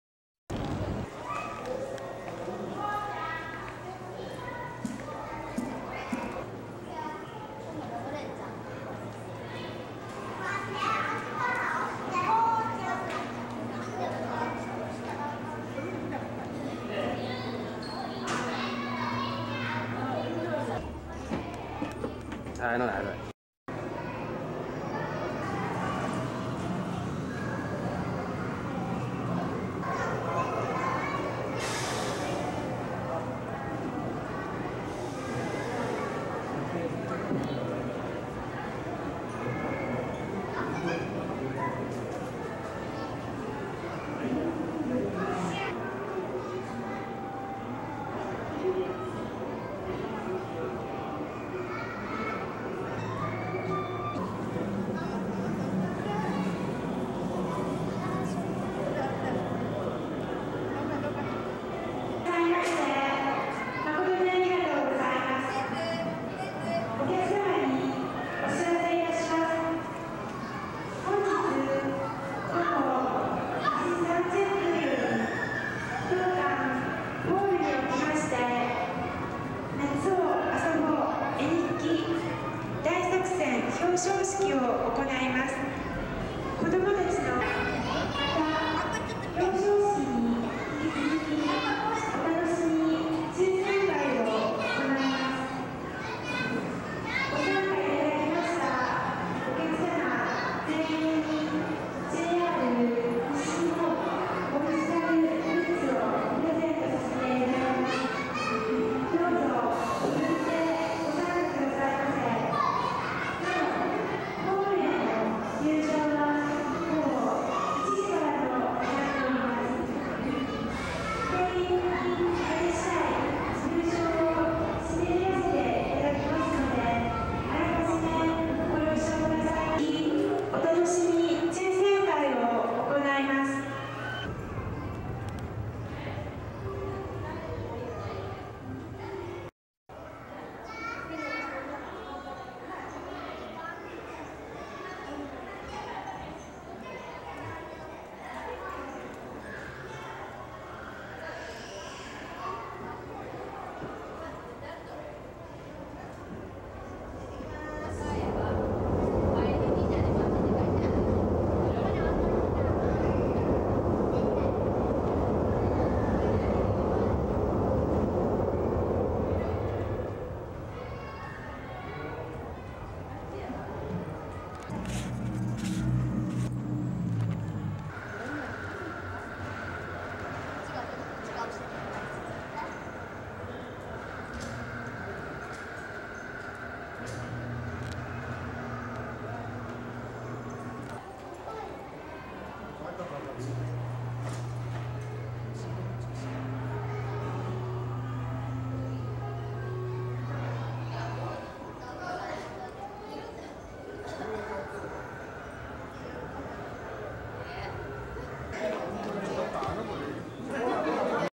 大阪 交通科学博物館 (1996年) 当時の交通科学博物館
大阪 交通科学博物館 (1998年) 平成10年9月27日
展示車両や鉄道模型パノラマ室のデモンストレーション運転の様子

交通科学博物館 大阪市 環状線弁天町駅隣接
施設の老朽化のため2014年４月６日閉館する
多くの人に惜しまれつつ５２年の歴史に幕を下ろし
実物車両２１両と収蔵資料約６万３０００点のうち
主なものは２０１６年春に京都市にオープンをした
京都鉄道博物館に移された